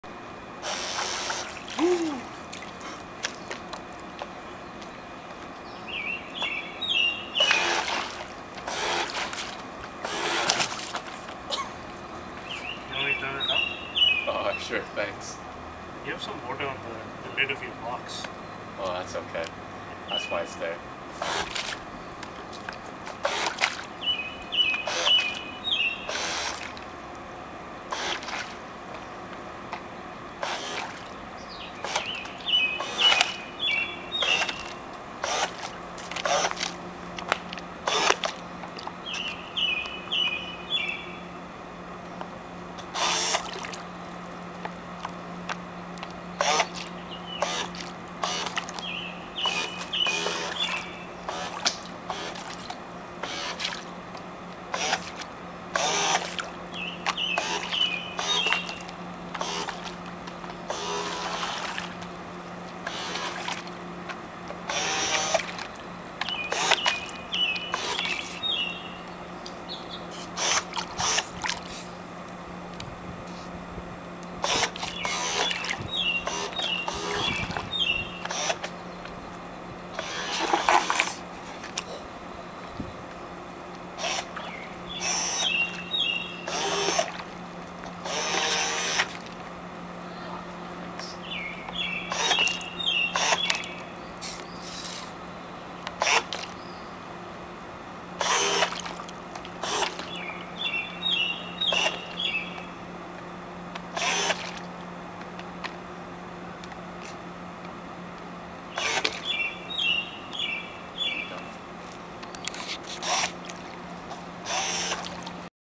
ME 218C RC boat
First on-water remote-controlled testing of our boat.

Bang-bang control with fairly "torquey" motors in a small pool meant we couldn't hold down the throttle. And we were cautious because we didn't have a cover on.

One of the shaft couplers came off at 0:48.